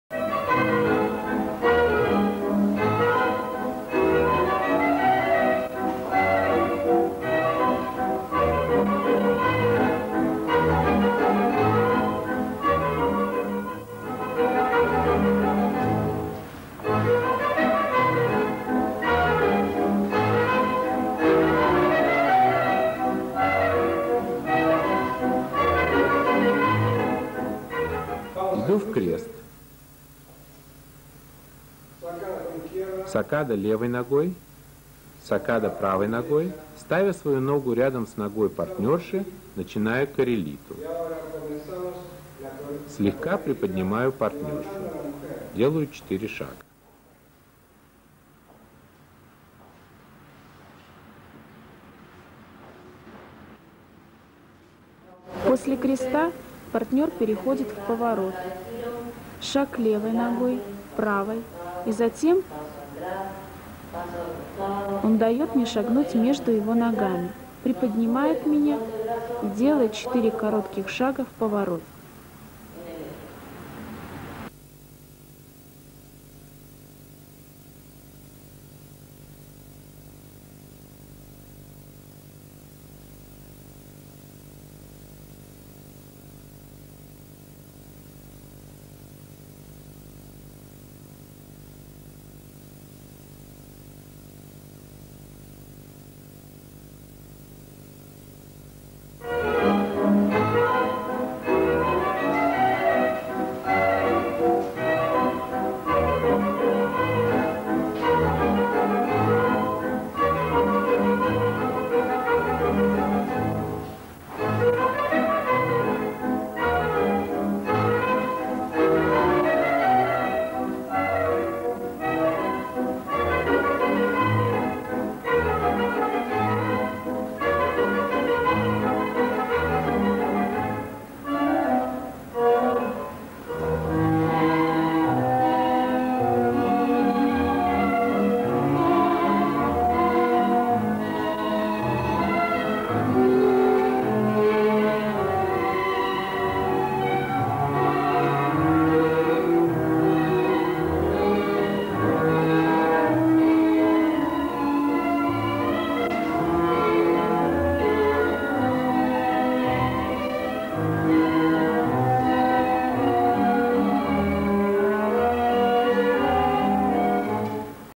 Танго-вальс. Урок 14. Сакады с пробежкой
Комментарии партнёра:
Вывожу в крест, Из креста вывожу в очо и делаю сакаду левой ногой, энтраду правой ногой. Ставя свою ногу рядом с ногой партнёрши начинаю коррелиту. Слегка приподнимаю партнёршу. Делаю четыре шага.

Комментарии партнёрши:
После креста партнёр переходит в поворот. Шаг левой ногой. Правой. И затем он даёт мне шагнуть между его ногами. Приподнимает меня и делает коротких четыре шага в поворот.